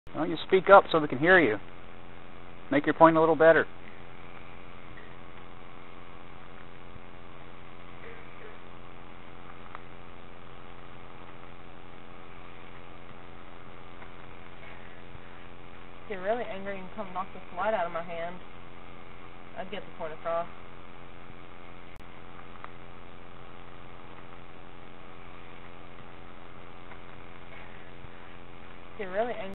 Big Woods Cemetery EVP - voice at cattle guard?
Repeats once after initial play.